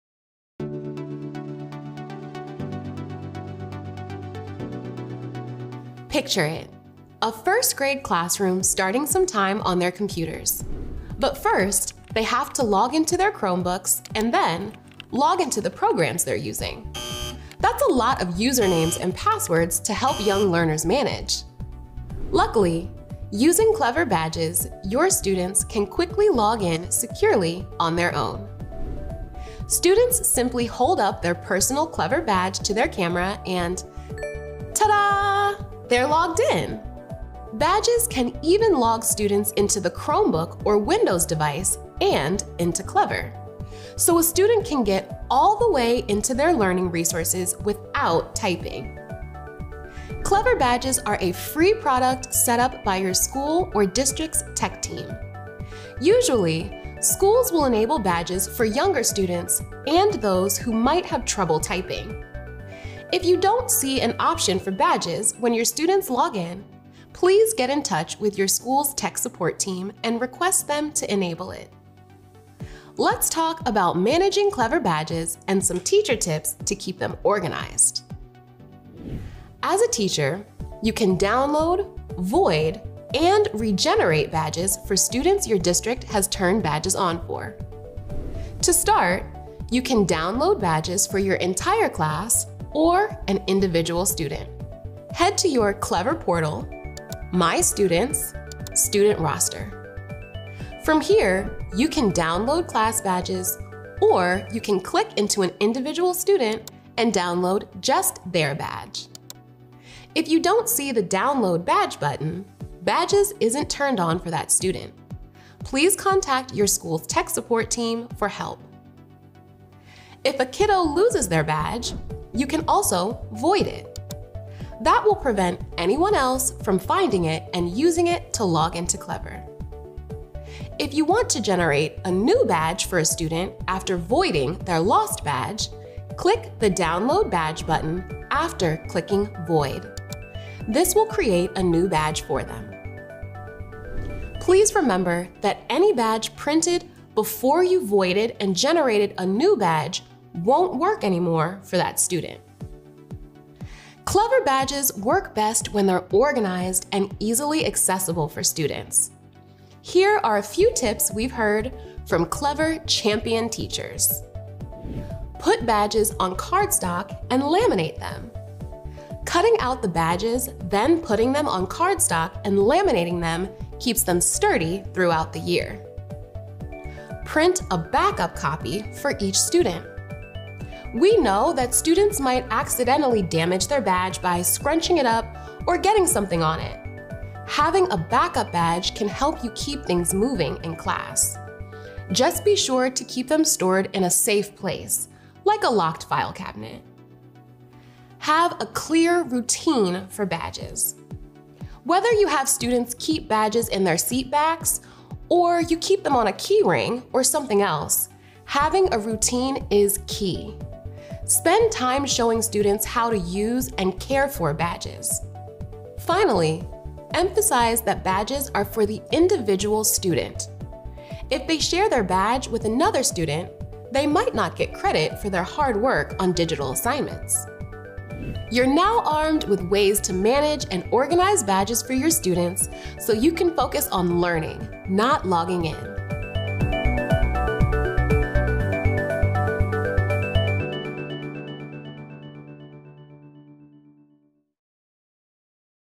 Managing Clever Badges
Clever Badges are a great way to get your students quickly and securely logged into the Clever account. Learn how to easily download, void, and print Clever Badges straight from your Clever Portal.